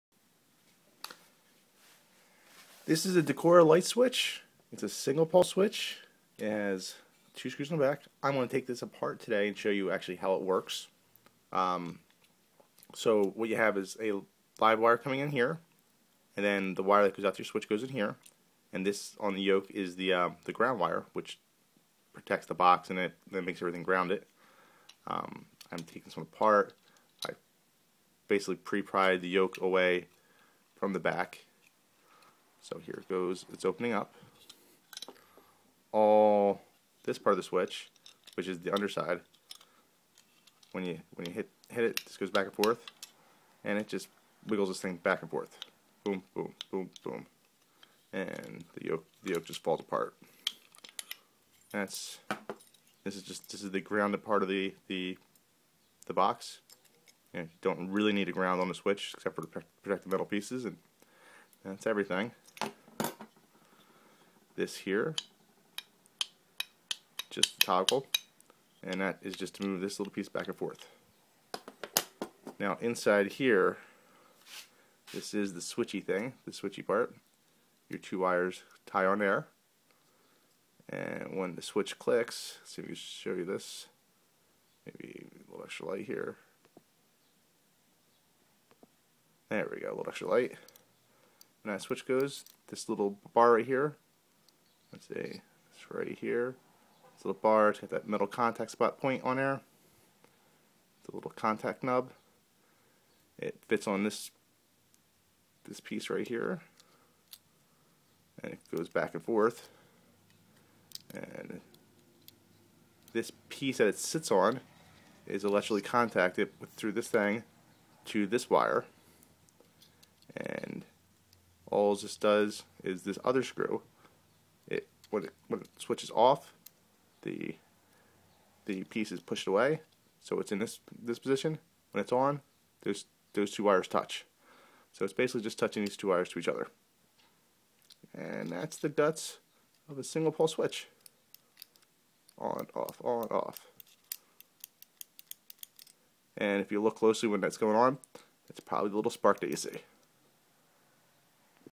Broken Light switch- How a square light switch works
What is inside your light switch. In this video I take apart a single pole Leviton 5601-w  light switch. With the switch opened up it is easy to understand where to hook up your wires. The grounding yoke is removed and you call also see what part of the light switch is grounded.

Amperage: 15 Amp - Voltage: 120/277 Volt - HP Rating: 1/2HP-120V 2HP-240V-277V - Max. Amperage: 12 Amp - Actuator Material: Thermoplastic - Body Material: Thermoplastic - Strap Material: Steel - Color: White - Standards and Certifications: UL/CSA - Thermoplastic actuat